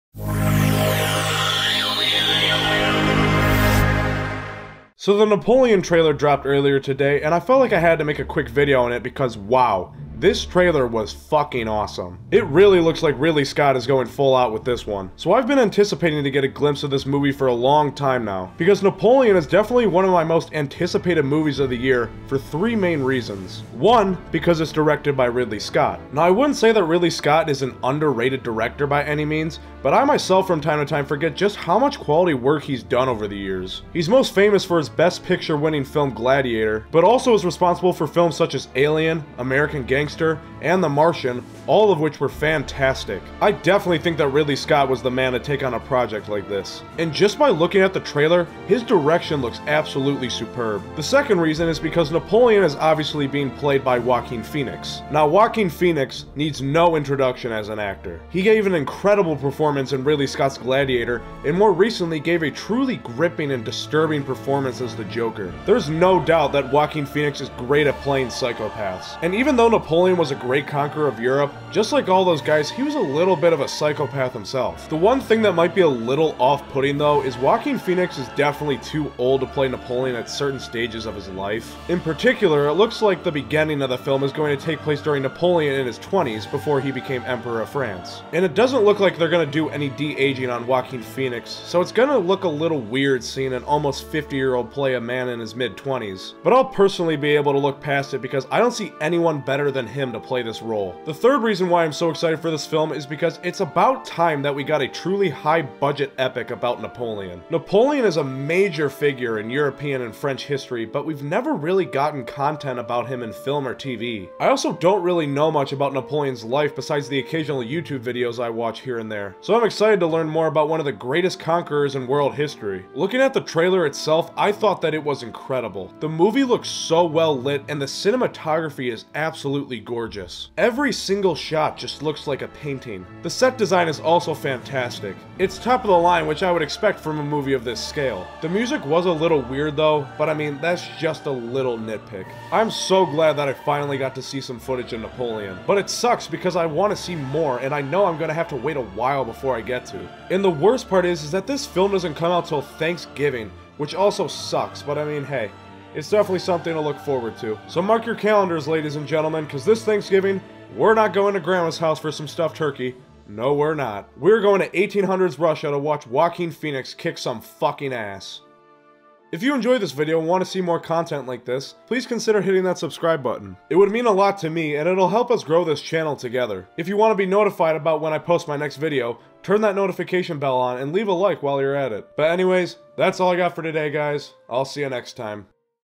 The Napoleon Trailer Is Fantastic
The Napoleon trailer just dropped and it was awesome.  Here are some of my quick thoughts about the trailer